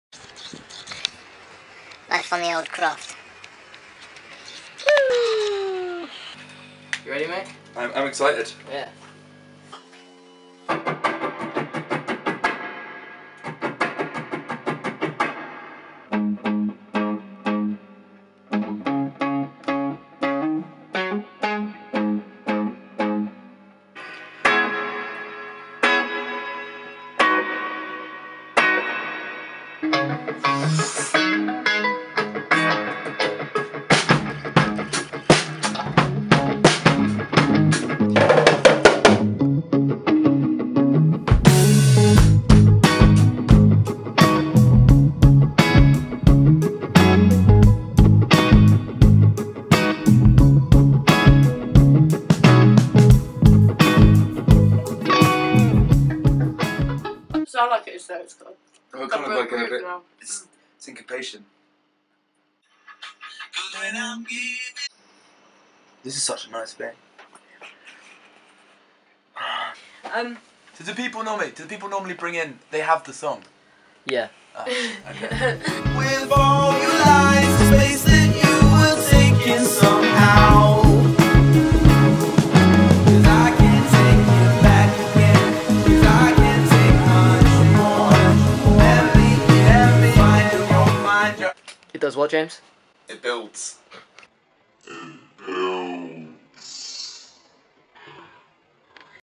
NineTree Sessions 2016 - James Humphrys
This time its Mr "Humphrys" and his time in the studio with LaBandana and some friends :)

Check him out on  these internet pages!!

More videos to follow

Peace!